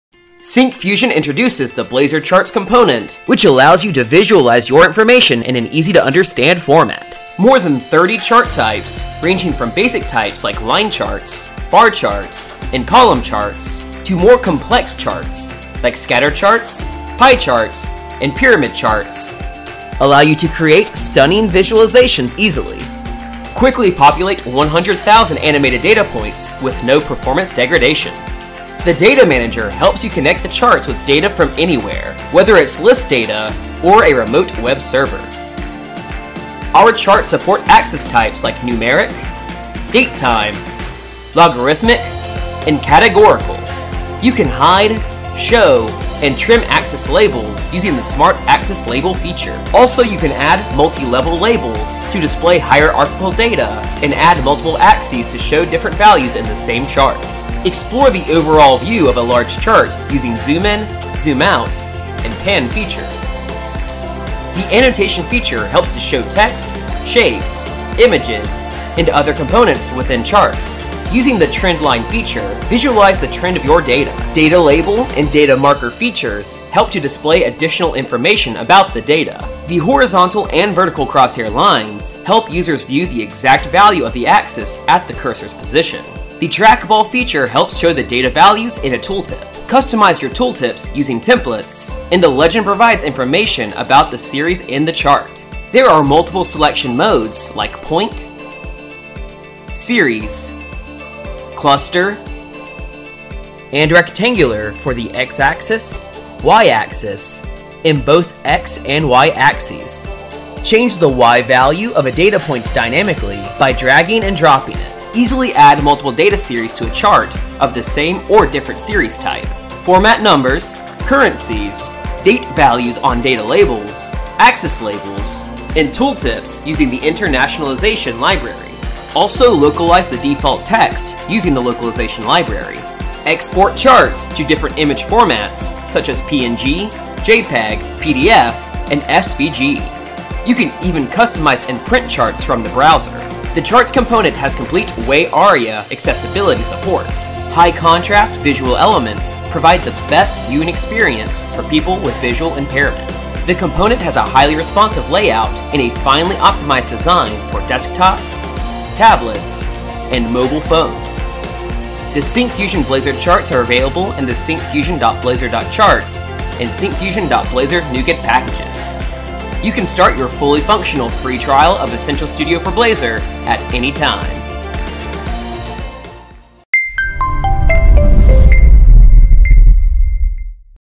Overview of Blazor Charts Component
Let’s visualize the comparative information in an easily understandable format, by plotting them against the respective axis using the Blazor Charts component of Syncfusion. More than 30 types of Charts are available. They range from line to financial charts and cater to all the charting scenarios.

The high performing quality of Blazor Charts helps you to render large volume of data quickly. It also comes with many other features like zooming, panning, tooltip, crosshair, trackball, drill down, and events to make it more interactive.

In this video, you will see an overview of the Blazor Charts component, which is available on all other major web platforms: JavaScript, Angular, React, Vue, ASP.NET MVC, and ASP.NET Core.